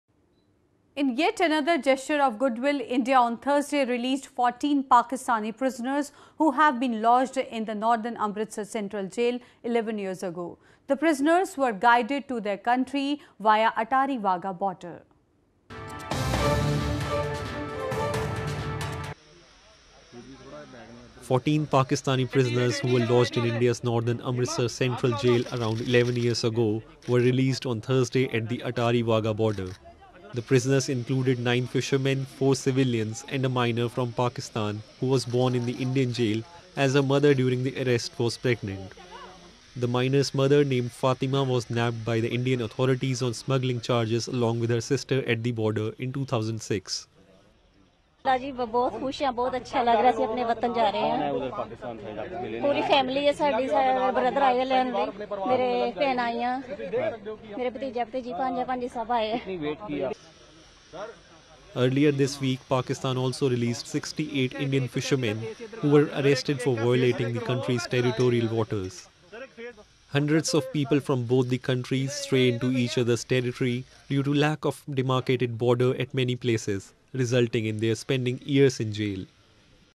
India News (Nov 02, 2017) - 14 Pakistani nationals released from jail in northern India
In yet another gesture of goodwill, India on Thursday released 14 Pakistani prisoners who have been lodged in the northern Amritsar central jail 11 years ago. The prisoners were guided to their country via Attari-Wagah border.